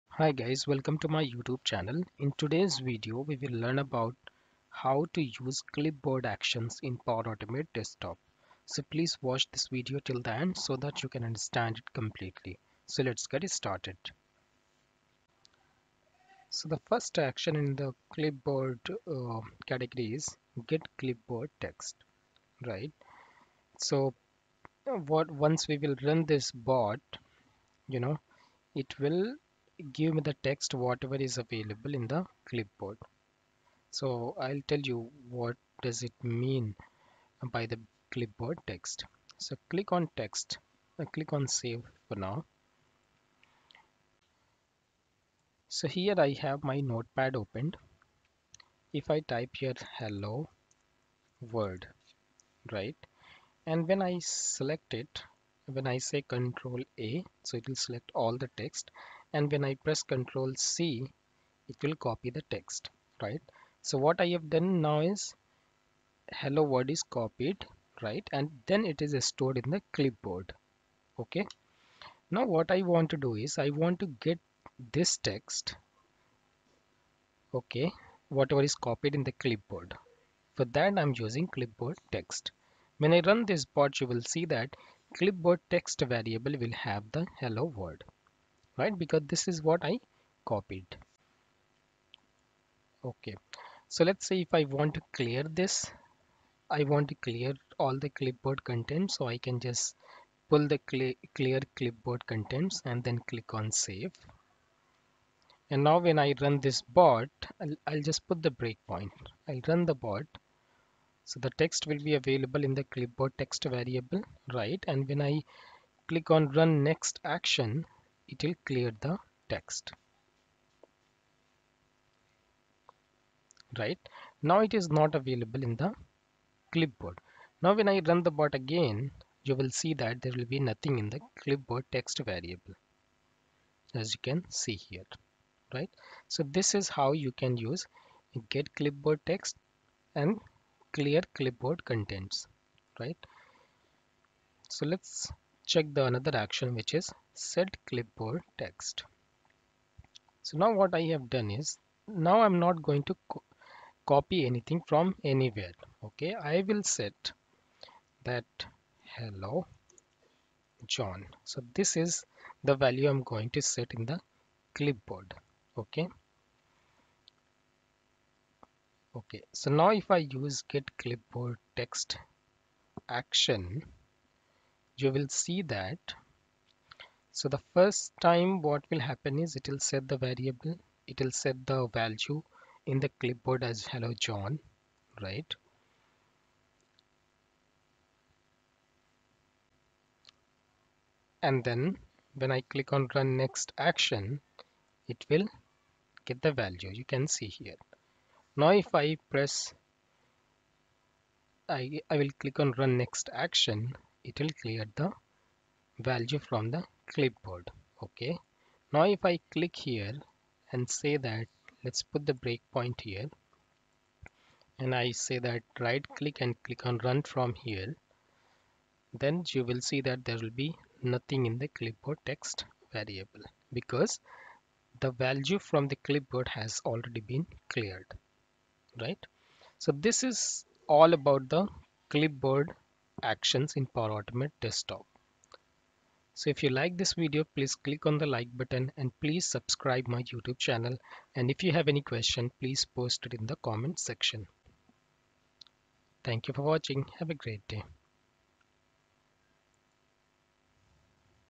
Clipboard Actions(Get/Set data in Clipboard,Clear contents from Clipboard) in Power Automate Desktop
In this Video, you will learn how to get and set text in Power Automate Desktop. Please watch this video till the end to understand it completely.